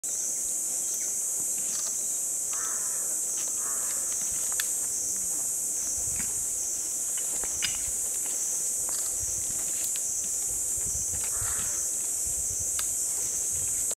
Komodo dragons and their prey...
A very rare moment witnessing two Komodo dragons eating their prey in wild..., Komodo Island, Indonesia, 15 August 2016